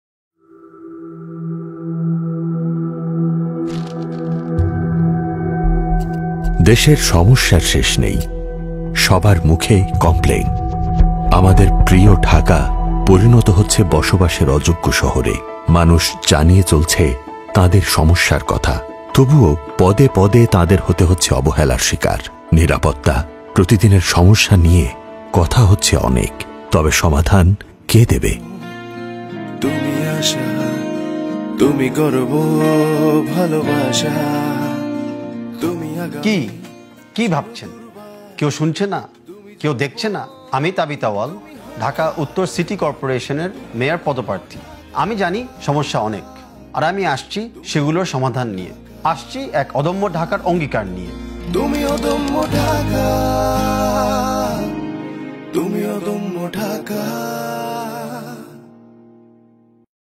সমস্যা অনেক, আমি আসছি সমাধান নিয়ে, আসছি এক অদম্য ঢাকার অঙ্গীকার নিয়ে-তাবিথ আউয়াল
আপনাদের মতামতের উপর ভিত্তি করেই প্রস্তুত করছি আমাদের নির্বাচনী ইশতেহার। আর কেউ শুনুক আর না শুনুক, আমি শুনছি।